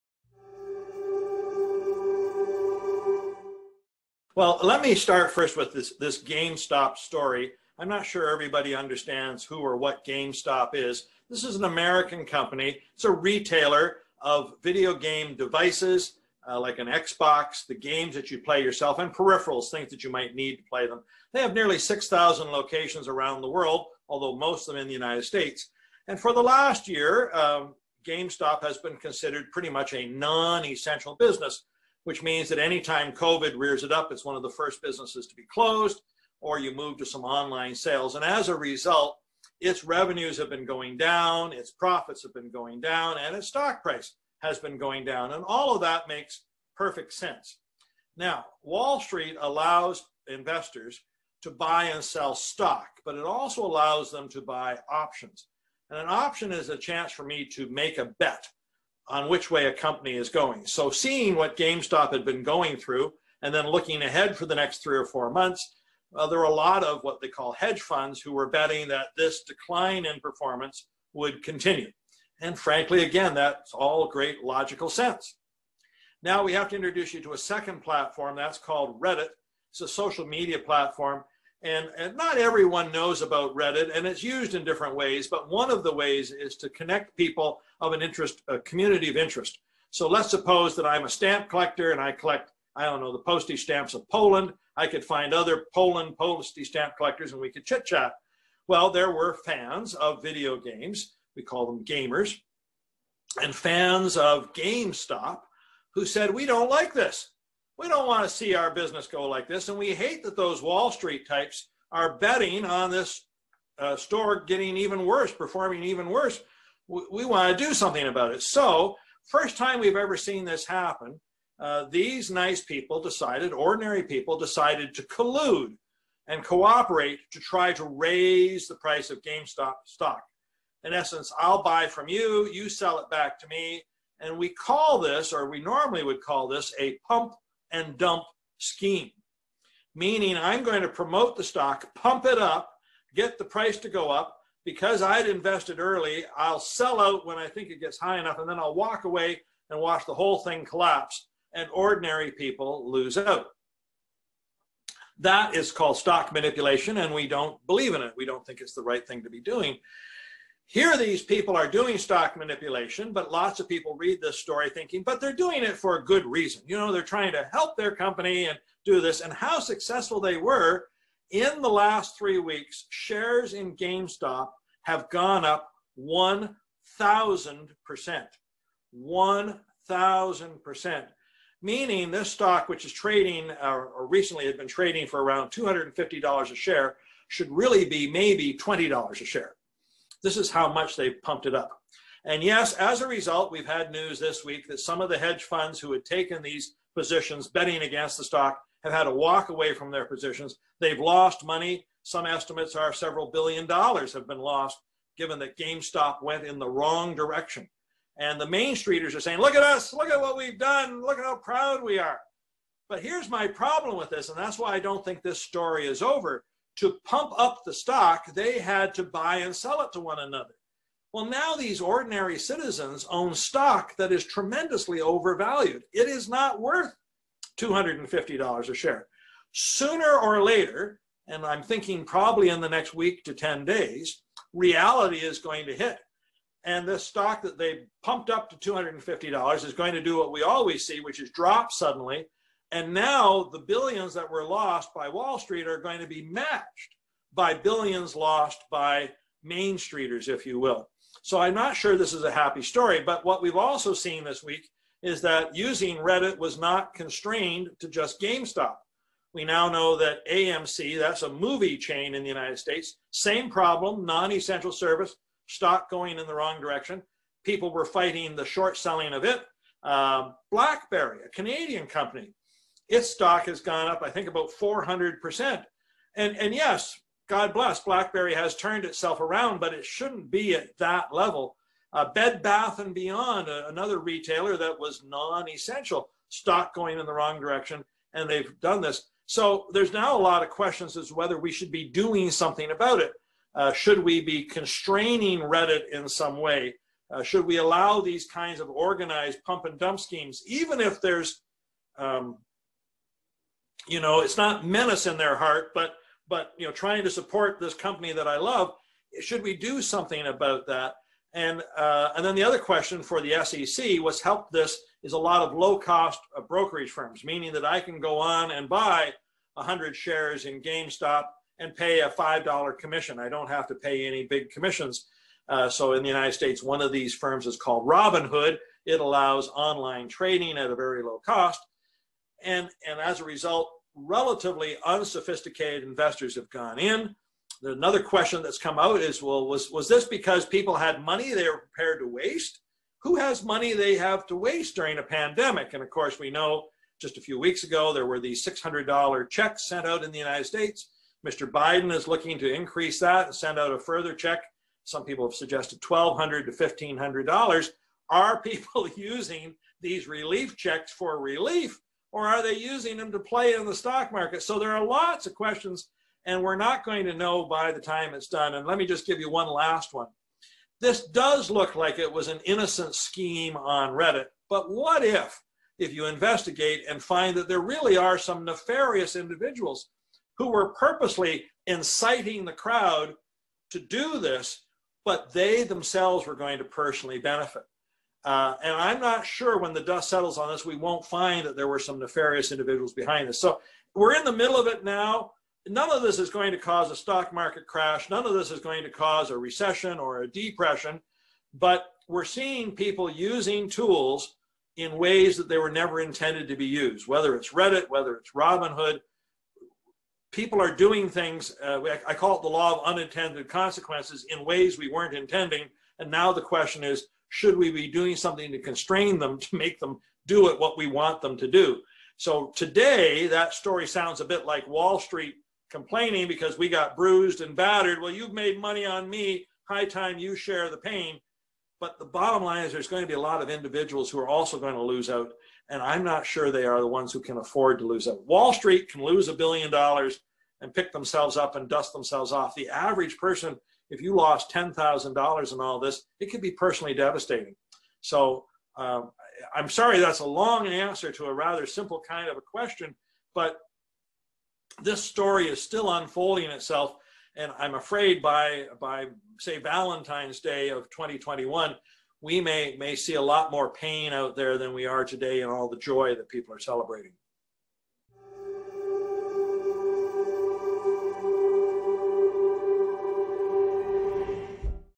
The Risk of Wall Street Bets | Knowledge Labs: In the Know
Wall Street Bets - have you heard of it? This subreddit has been taking the markets by storm and has caused stocks for companies like GameStop, AMC, and BlackBerry, among others, to rise unexpectedly. Why is this happening? Associate professor Marvin Ryder gives us the full picture in this week's Knowledge Labs: In the Know.

Knowledge Labs: In the Know is a weekly web series where experts from the DeGroote School of Business offer their insights into current affairs.